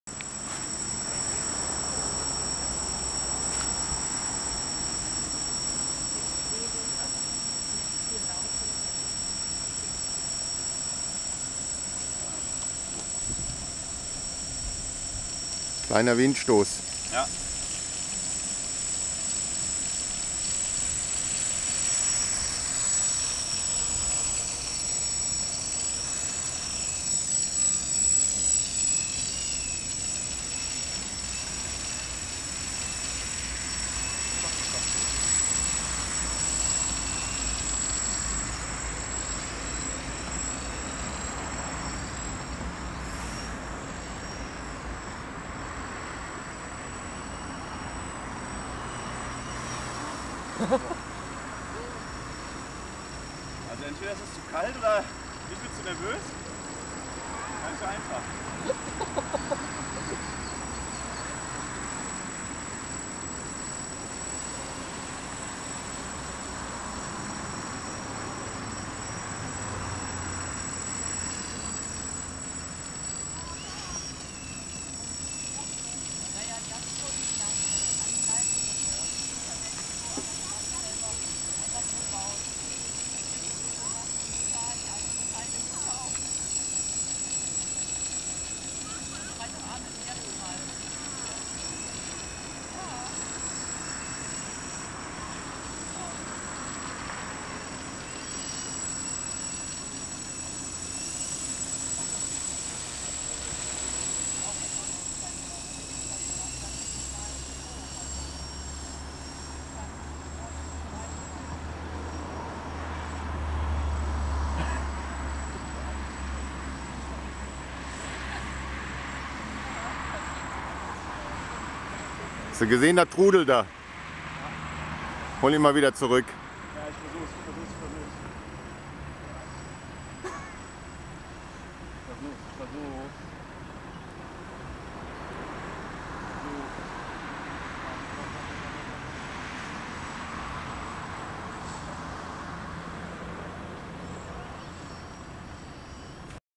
2012-01-29-Quadrocopter
Nachdem gestern wegen anderer Projekte kein weiterer Flug zustande kam, mussten wir heute bei kühlem Wetter noch einmal einen Versuch starten. Zuerst wurde noch einmal alles kontrolliert und die Kabel von den Leuchtdioden mussten noch einmal angelötet werden. Warm anziehen und dann noch mal raus auf den großen Parkplatz. Start und erste Flugübungen gingen noch ganz gut. In größerer Höhe trudelte der Copter dann aber deutlich und driftete vom Parkplatz weg, so dass Mark Probleme hatte, ihn wieder kontrolliert zurück zu holen. Letztlich ist er dann gegen die Ecke vom Grillmastergebäude geflogen und zwei Rotorblätter waren hin. Eine kurze Episode, die die Erkenntnis gebracht hat, dass wir noch viel üben und das Ding besser verstehen lernen müssen.